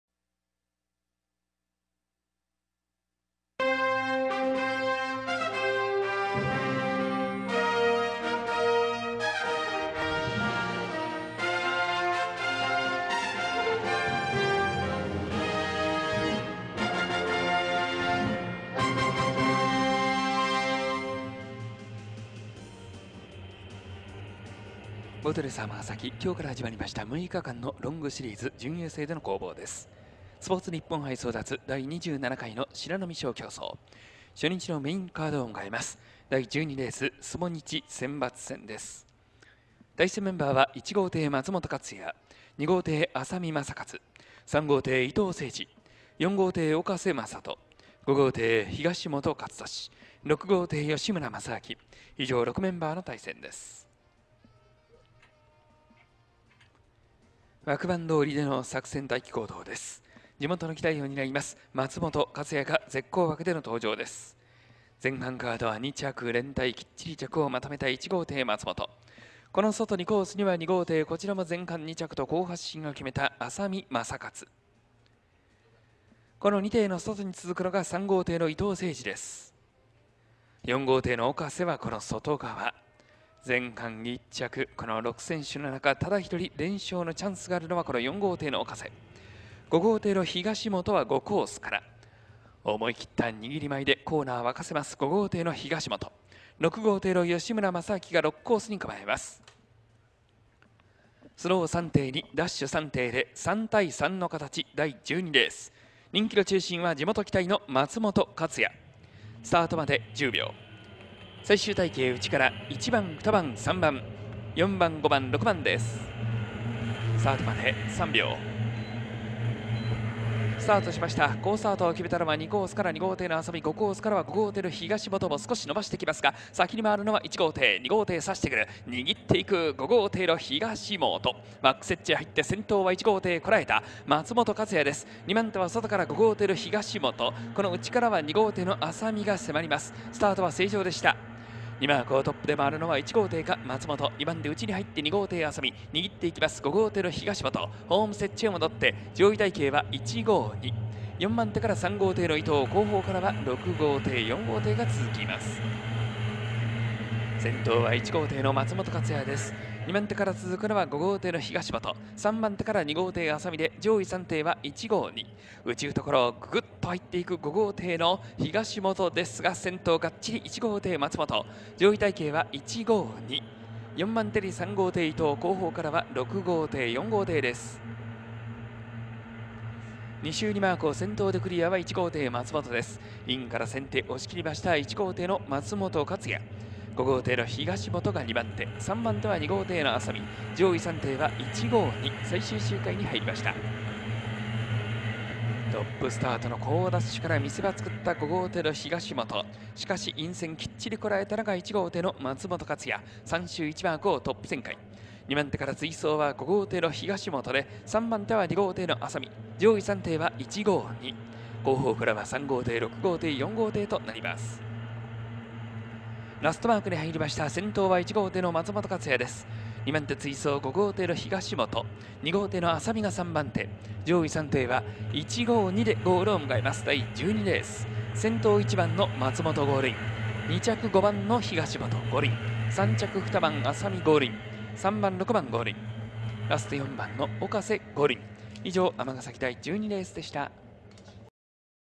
【ボートレース/競艇】尼崎 スポーツニッポン杯争奪 第27回しらなみ賞競走 スポニチ選抜戦 初日 12R 2017/6/27(火) BOAT RACE 尼崎
１号艇　松本　勝也
２号艇　浅見　昌克
３号艇　伊藤　誠二
４号艇　岡瀬　正人
５号艇　東本　勝利
６号艇　吉村　正明
尼崎競艇場、スポーツニッポン杯争奪 第27回しらなみ賞競走 スポニチ選抜戦 初日 12R！このレースを制して、シリーズを引っ張るのはどの選手か！

【関連動画】